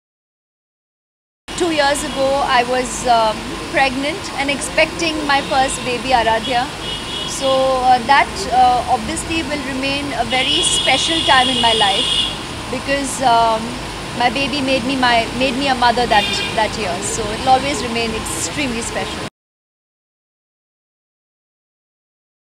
Aishwarya Rai Talks About Her Special Moment In Her Life - Aishwarya Rai Birthday 2013
Bollywood actress Aishwarya Rai Bachchan shares her very special moment in her life with her fans on her 40th birthday. Watch the video to find out.